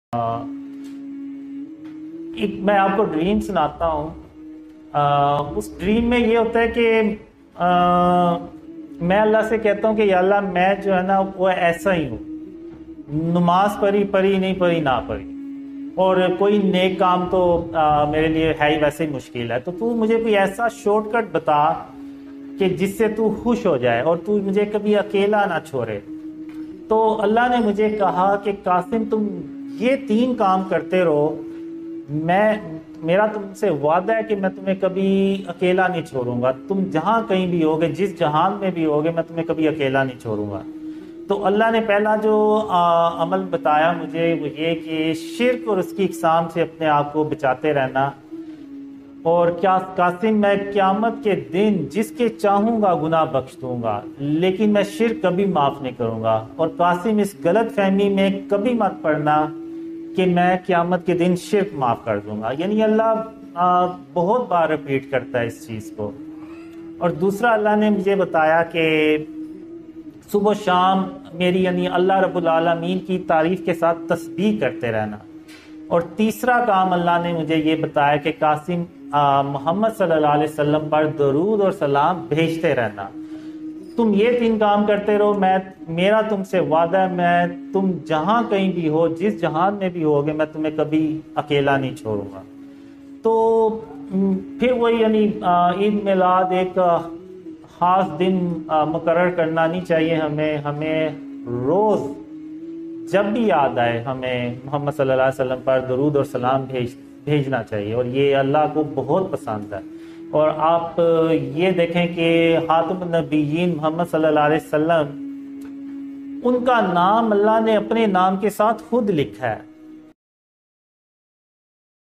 3 Hal yang harus dilakukan muslim agar Allah SWT bersama kita
Selamat datang di Papabrand channel! Di video kali ini, kami akan membahas mimpi yang luar biasa dari Muhammad Qasim.
dimana ALLAH SWT dan Rasulullah SAW √memberikan kabar gembira bagi umat islam
√ada juga peringatan petunjuk dan tanda tanda akhir zaman
√Qasim juga diperintahkan oleh ALLAH SWT dan Rasulullah SAW untuk membagikan mimpi-mimpi ke semua orang.

Mimpinya tentang
•Bentuk syirik di zaman modern
•Cara menghindari syirik
•Terkait Pakistan
•Terkait Akhir Zaman